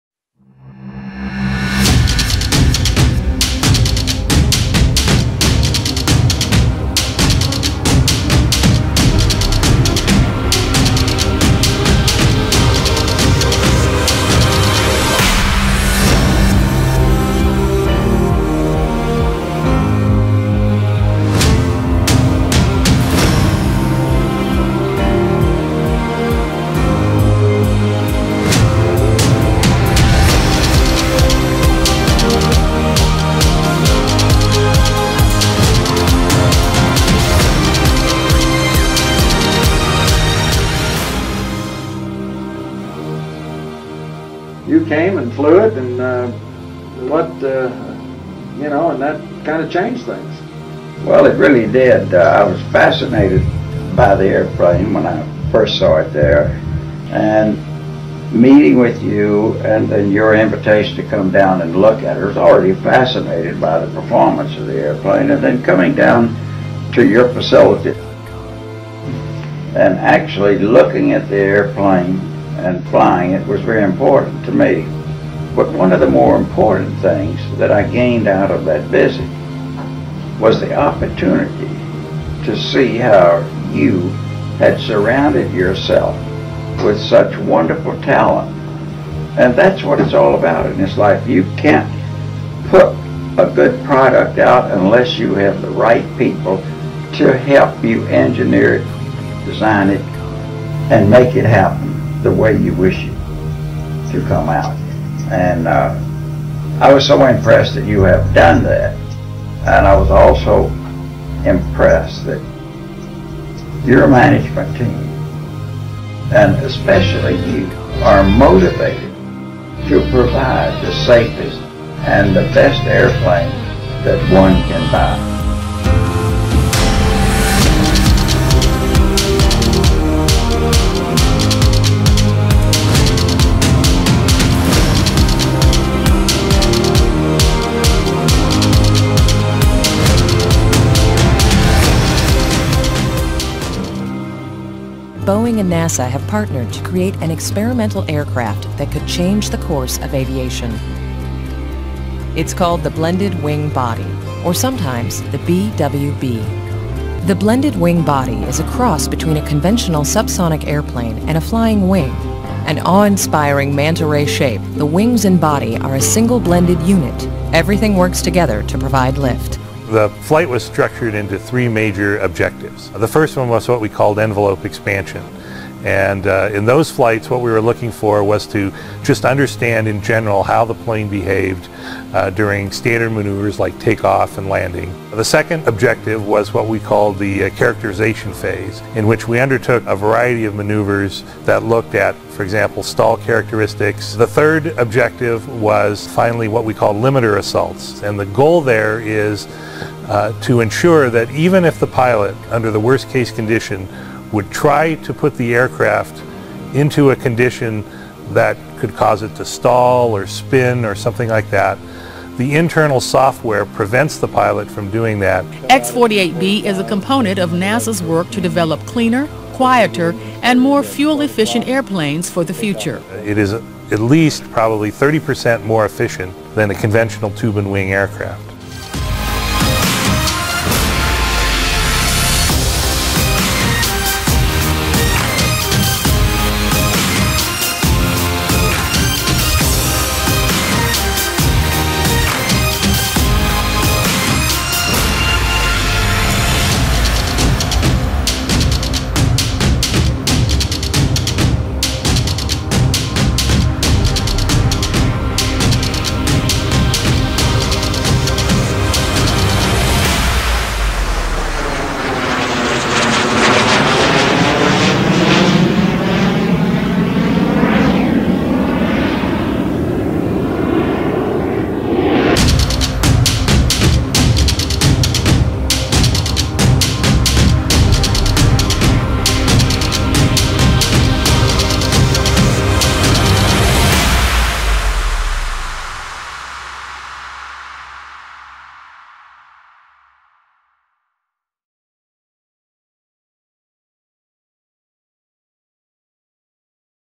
Promotional Video for Maverick Jet's Upcoming Jet - MaverickONE
Animation, video editing and audio mixing by ThinAirStudios. Interior stills (other than cockpit) by ThinAirStudios.

LeaderJET footage courtesy of Maverick Jets.

BWB footage courtesy of NASA and Boeing.